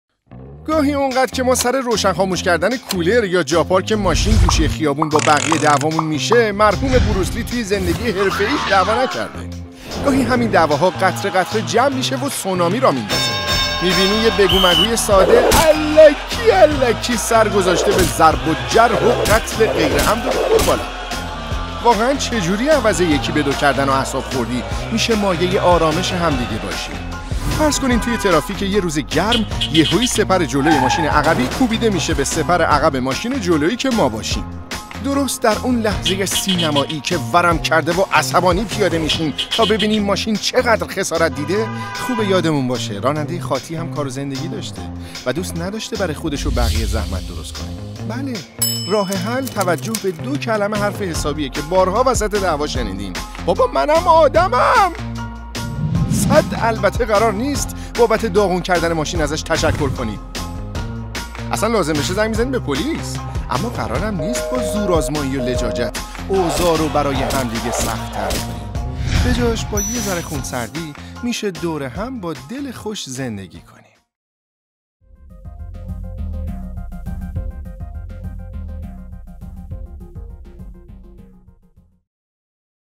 اینقدی که ما سر خاموش کردن کولر دعوا میکنیم مرحوم بروسلی تو زندگیش دعوا نکرده😂😂
با یک مقدار خونسردی میتونیم دور هم با دل خوش زندگی کنیم😍

 فیلم کوتاه ایرانی
موشن گرافیک کوتاه آسیب های اجتماعی
واحد هنرهای تصویری حوزه هنری
سایر اطلاعات فیلم کوتاه آسیب های اجتماعی
مدت زمان: 2 دقیقه
کشور: ایران
زبان: فارسی